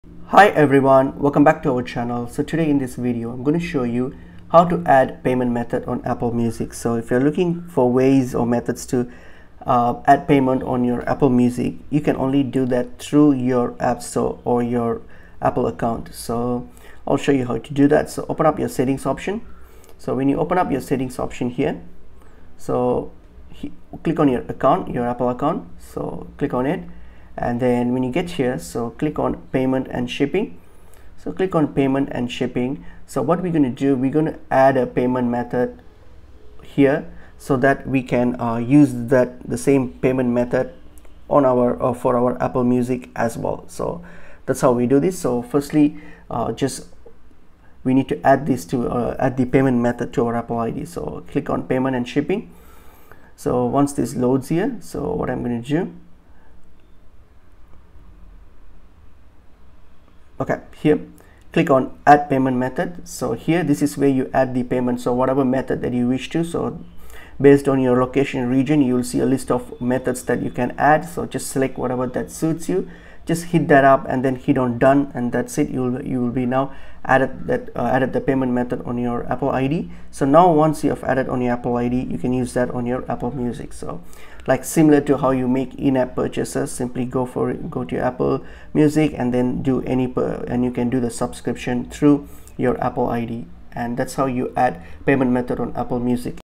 How to add payment method on Apple Music (BEST METHOD)
Having trouble adding a payment method on Apple Music? Look no further! In this video, we'll show you the BEST method to easily add a payment method to your Apple Music account.

Whether you're a new user or have been using Apple Music for a while, adding a payment method is a crucial step in enjoying your favorite tunes. Follow along as we guide you through the process step by step. Don't miss out on all the great features of Apple Music. Watch now and start jamming to your favorite songs!